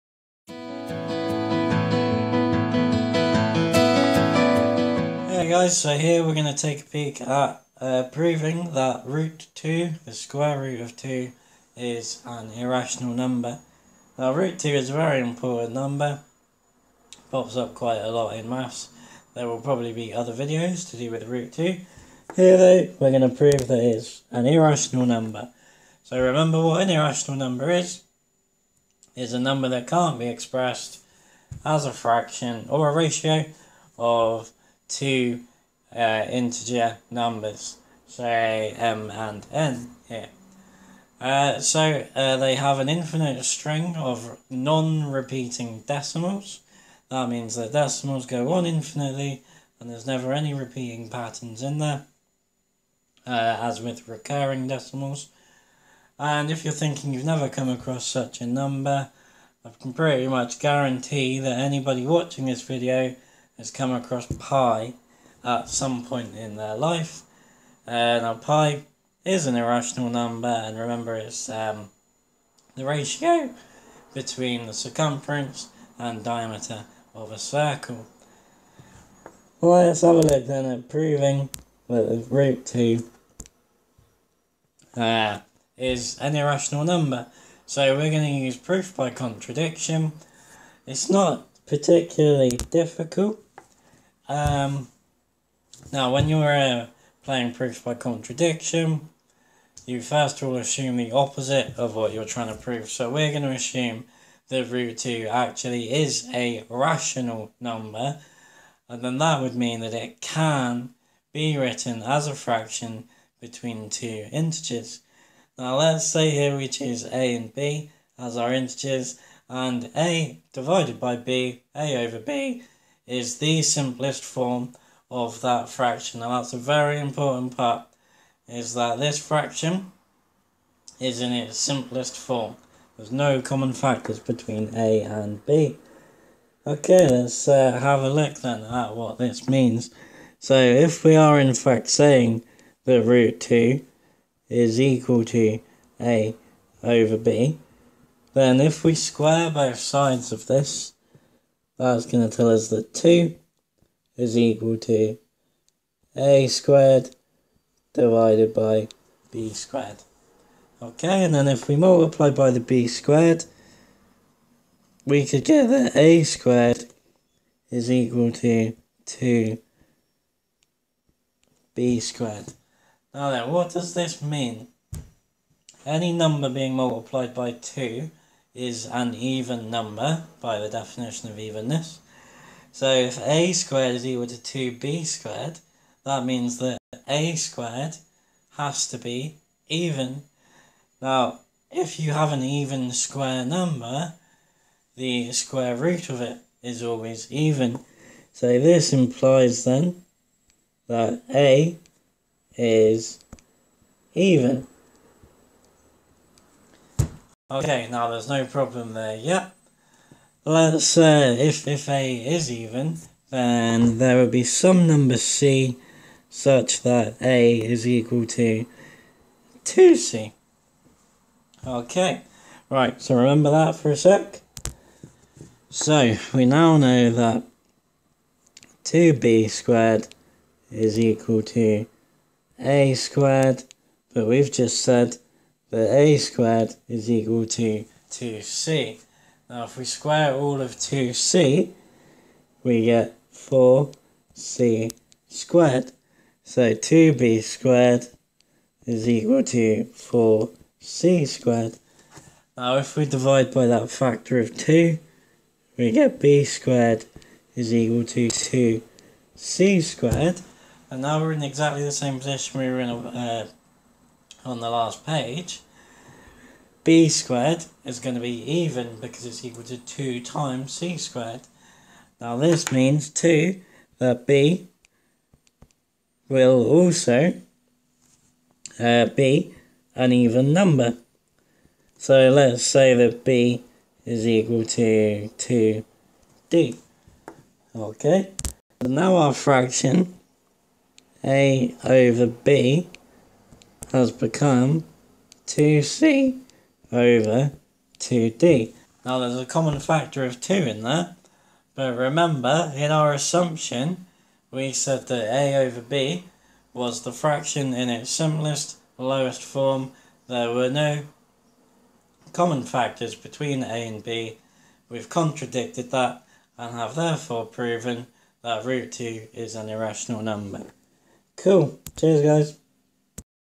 Proof by Contradiction Root 2 Is Irrational - A Level Maths
Here, we have a very famous proof by contradiction that root 2 is an irrationality number!

Proof by contradiction is my favourite type of proof, and you begin by assuming the opposite to the conjecture is true, and then shoe that that leads to a contradiction.

For the PROOF chapter of the NEW A LEVEL MATHS Year 2 for AQA, Edexcel, OCR and OCR MEI.